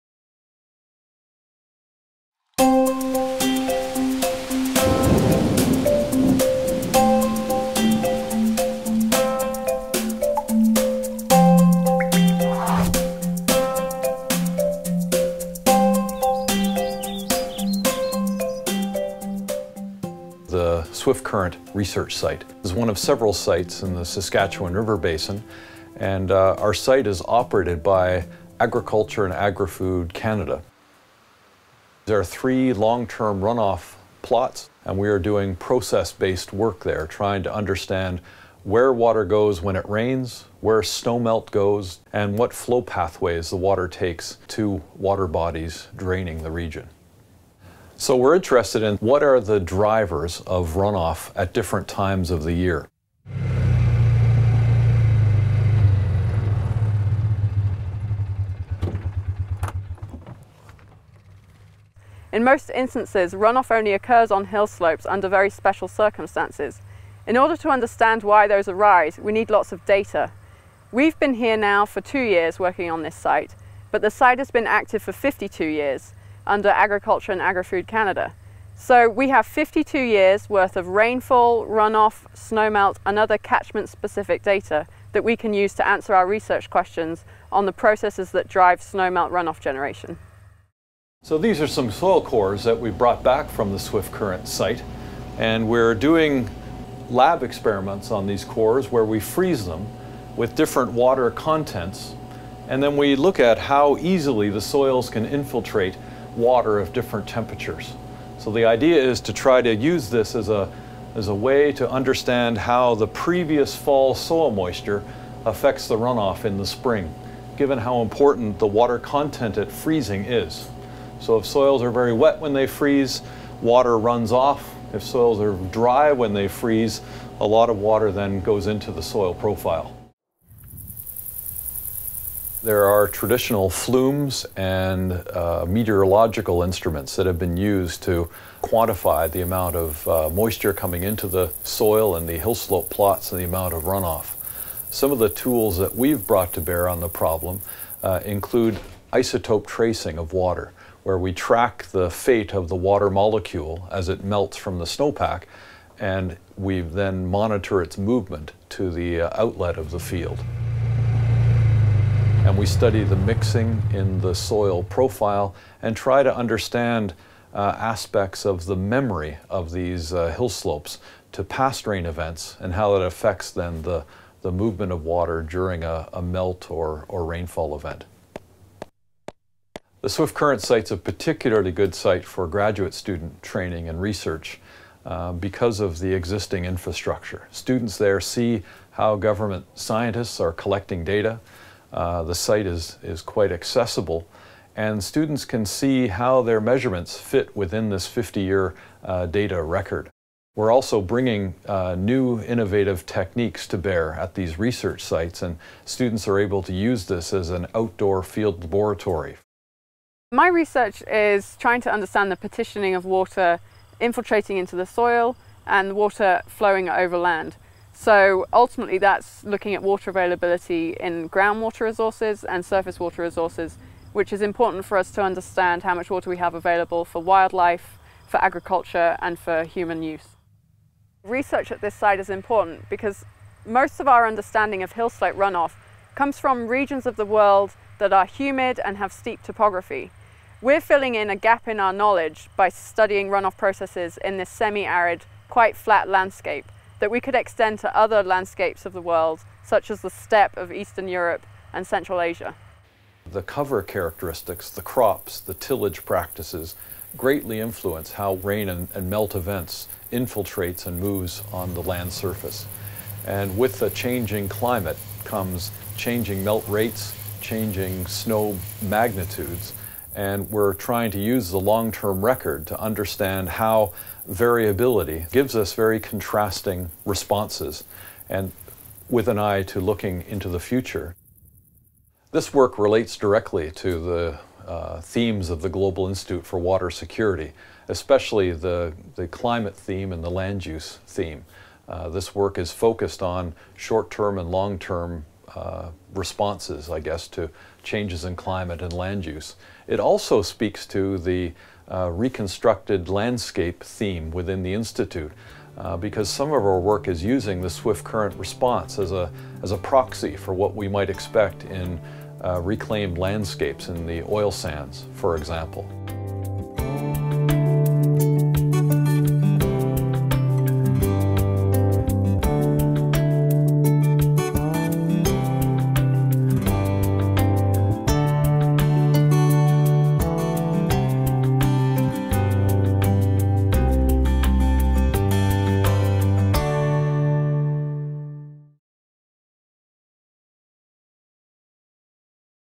Swift Current Research Site - Saskatchewan River Basin
University of Saskatchewan researchers are studying what the drivers are for water runoff at different times of the year in this prairie landscape in southern Saskatchewan. This Global Institute for Water Security research site is operated by Agriculture and Agri-Food Canada.